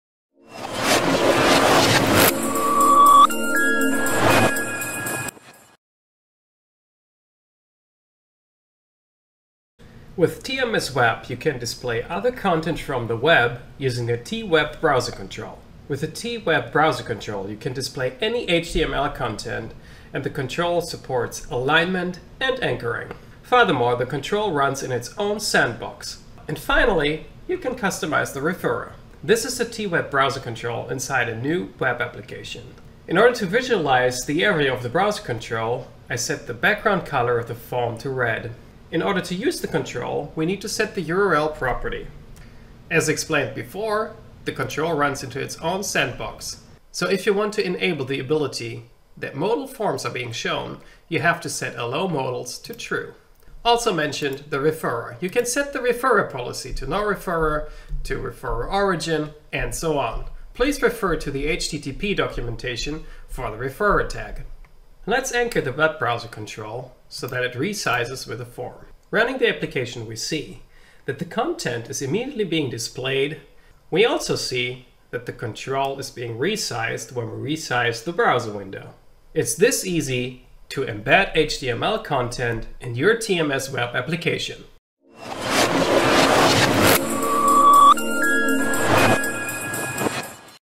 TMS WEB Core: TWebBrowser
Discover how TMS WEB Core makes adding web content to your apps a breeze! Learn to display HTML easily with the TWebBrowserControl. Customize settings like alignment and sandboxing effortlessly. Plus, see how to resize controls and make your apps dynamic.

ABOUT:
TMS software, established in 1995, is a software development company specialized in: